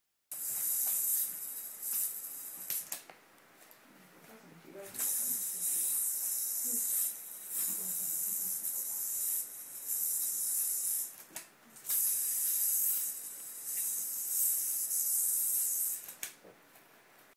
ヨーヨー少年の技
ラセレーションバインド、フラインググリーントライアングル、スーサイドキャッチをしました。